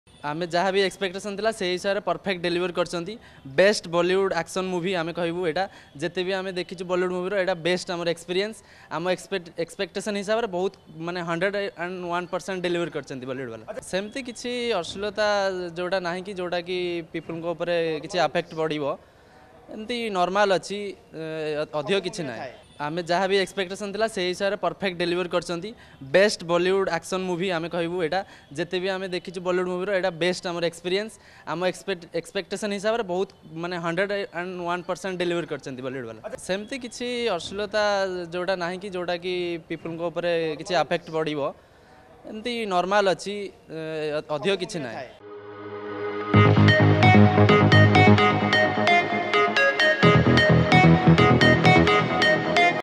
‘ବେଷ୍ଟ ଫିଲ୍ମ ହେଇଛି’ | Odisha Reporter
ବଲିଉଡରେ ସବୁଠାରୁ ବେଷ୍ଟ ଫିଲ୍ମ ହୋଇଛି ପଠାନ। ଯେମିତି ଲୋକଙ୍କୁ ଦରକାର ସେମିତି ହୋଇଛି ଫିଲ୍ମ ବୋଲି କହିଛନ୍ତି ପଠାନ ଫିଲ୍ମ ଦେଖିଥିବା ଦର୍ଶକ।